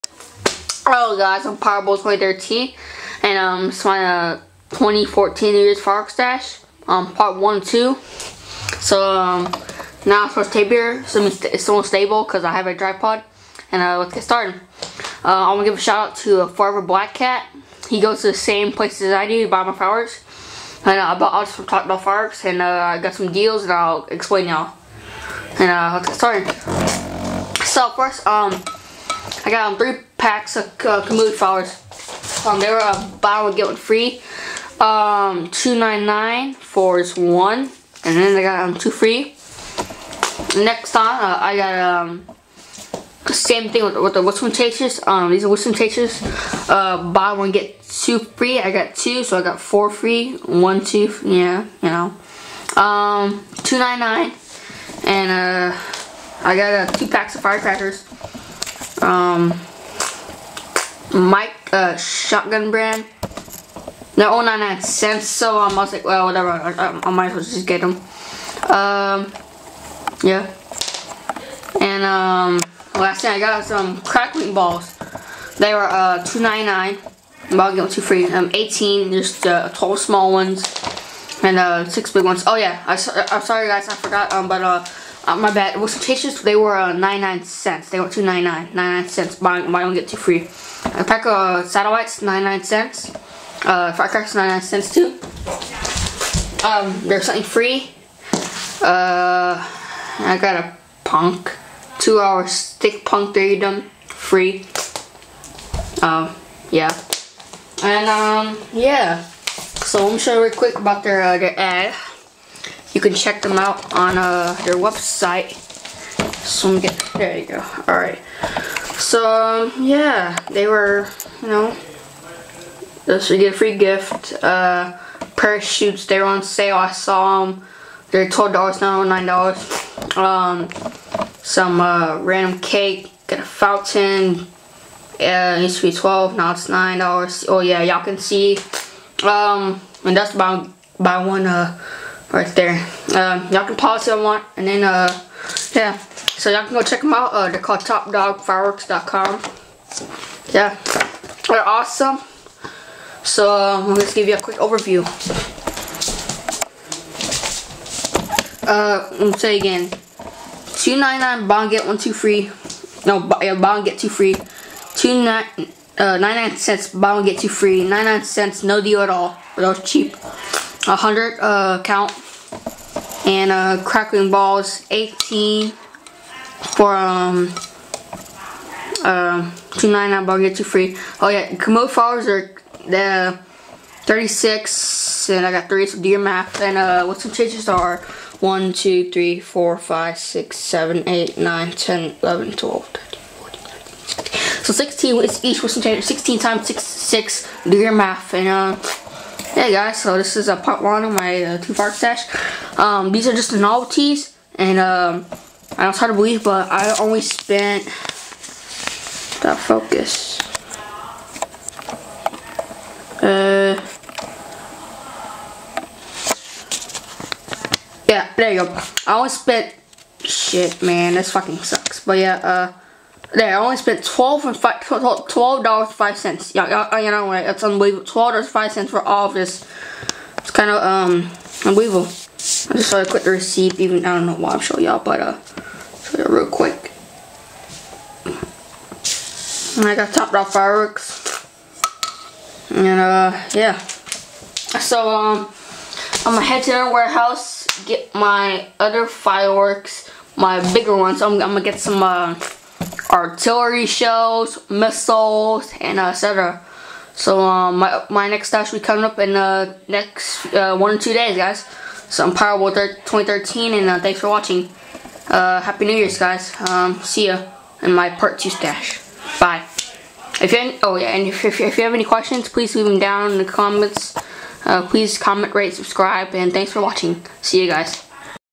2014 New Years Firework Stash Part 1 Of 2
Spent $12.05 This Is Part 1 Of 2.Gonna Make A Second Trip.Shout Out To xxFOREVERBLACKCATxx For Going To The Same Place As Me.